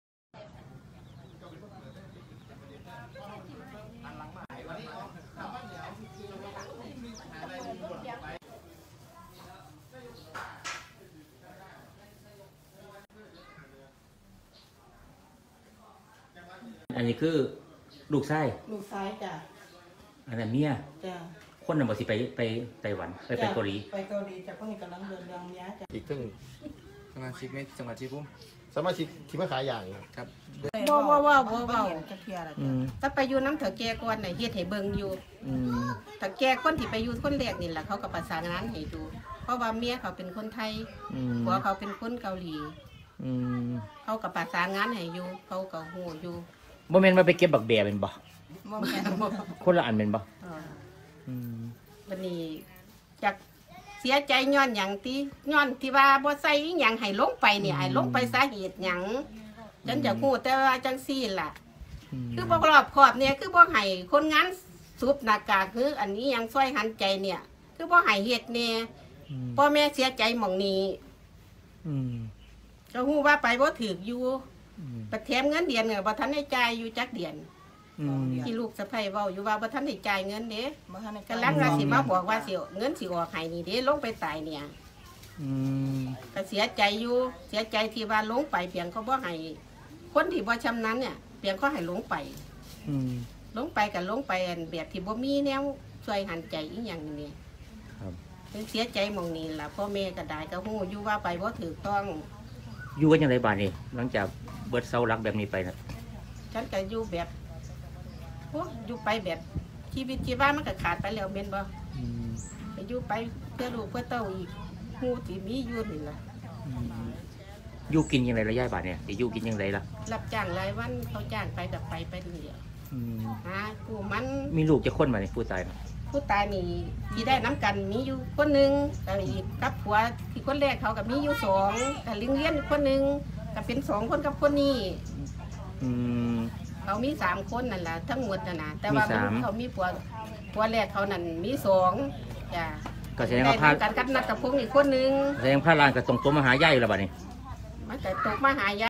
แม่และญาติแรงงานชาวชัยภูมิ 2 คนตายที่เกาหลีใต้ วิงวอนรัฐเร่งช่วยนำศพกลับบ้านเกิด
แม่และญาติแรงงานไทยชาวชัยภูมิ 2 คนเสียชีวิตจากเหตุสูดดมแก๊สพิษในถังใต้ดินที่เกาหลีใต้ วอนขอหน่วยงานรัฐที่เกี่ยวข้องและรัฐบาลไทย เร่งช่วยเหลือนำศพกลับมาบำเพ็ญกุศลที่บ้านเกิดโดยเร็ว เผยเฝ้ารอด้วยความหวังหลังไม่ได้เจอกันมานาน